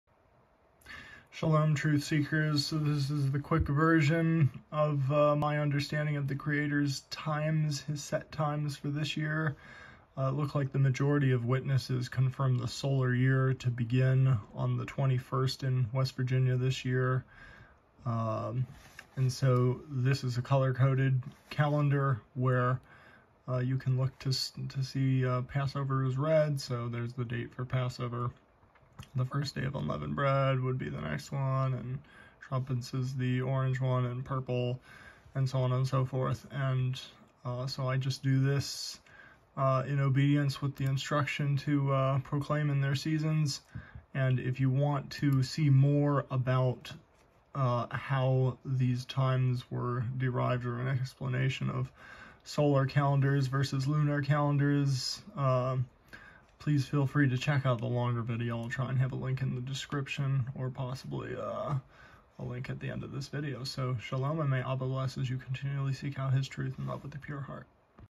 The Creator's Calendar 2022 (His Set Times per Genesis 1:14 & Leviticus 23)… short version
Be ware of the man made international date line, and don't let the "calendars" of men depart from the witness/confirmation of the heavenly luminaries per Genesis 1:14.